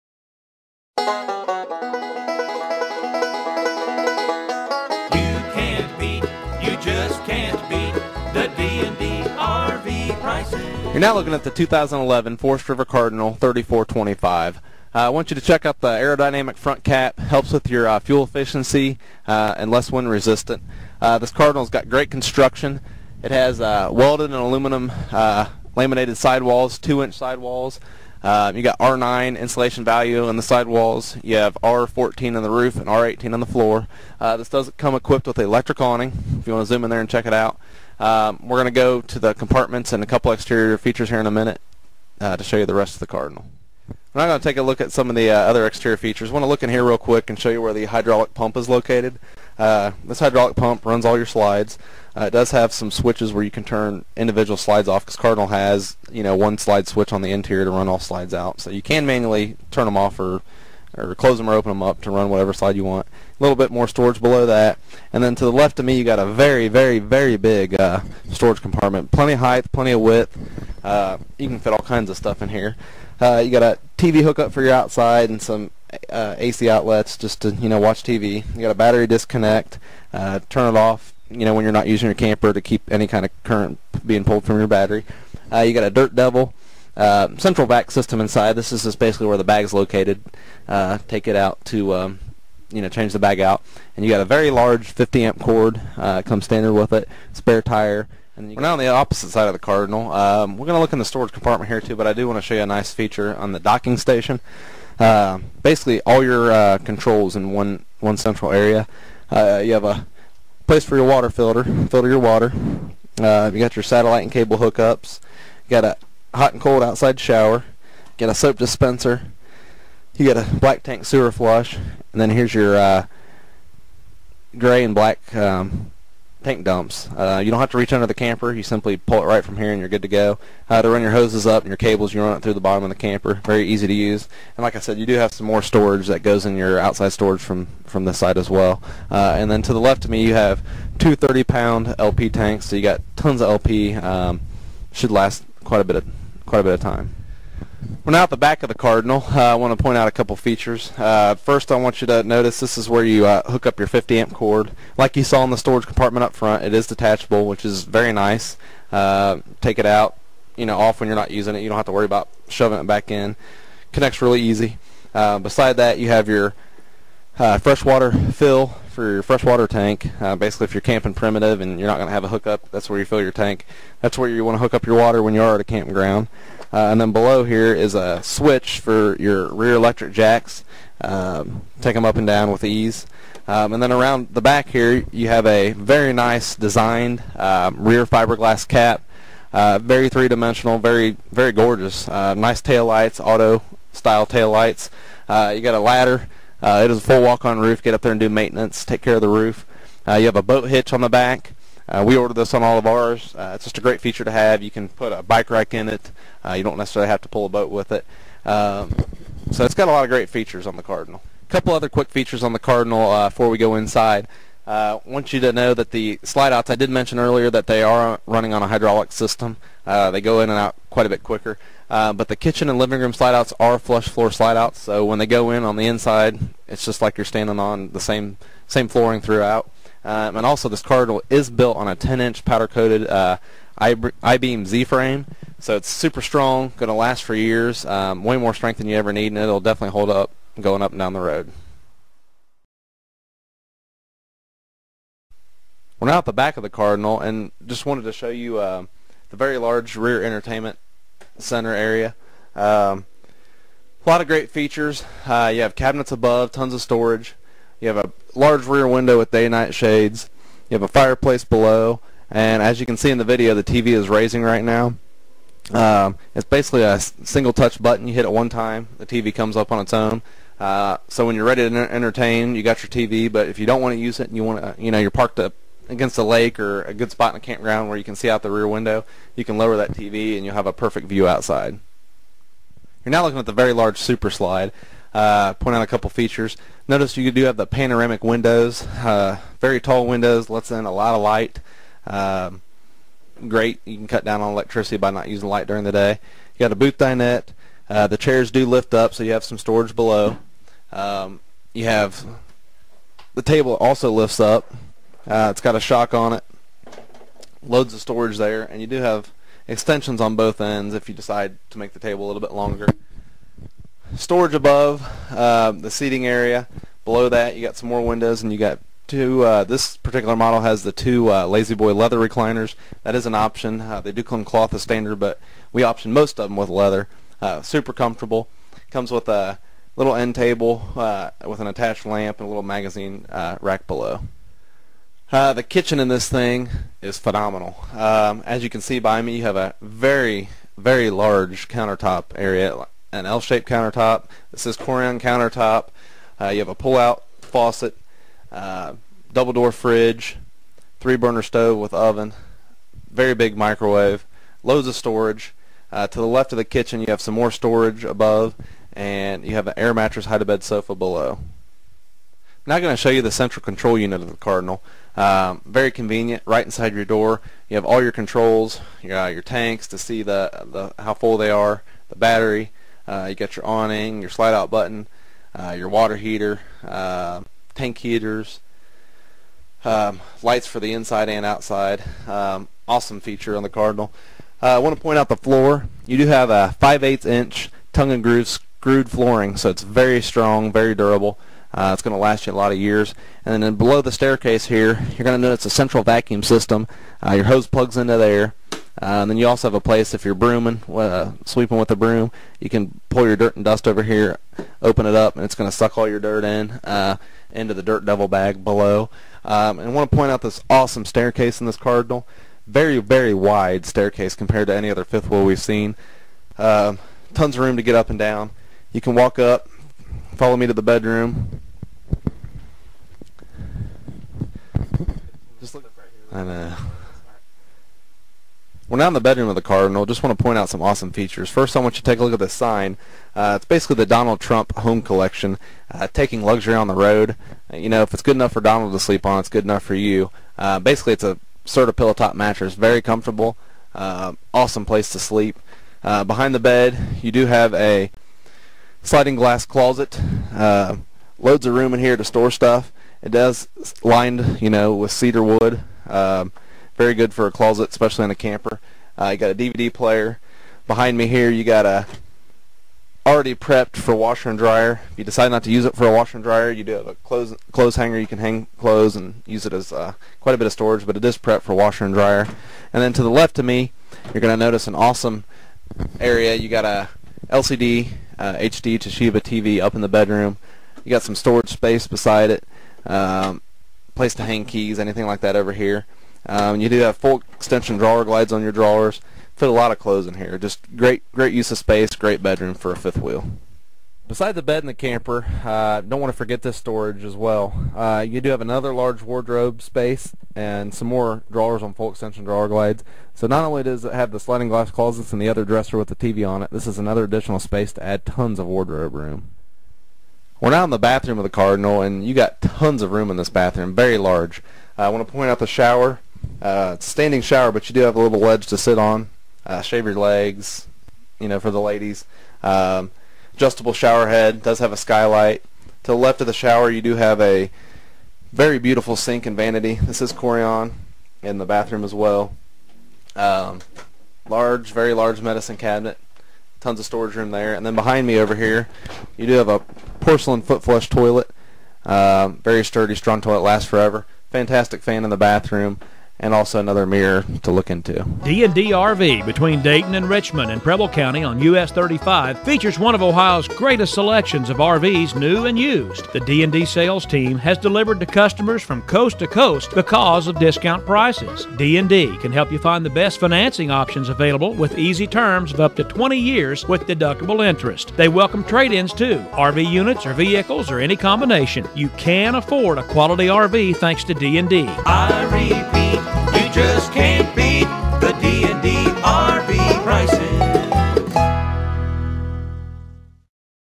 2011 Forest River Cardinal 3425RT Luxury Fifth Wheel at D&D RV
This is a 2011 Forest River Cardinal 3425RT rear living model fifth wheel. We are an RV Wholesale dealer with great inventory and even better pricing.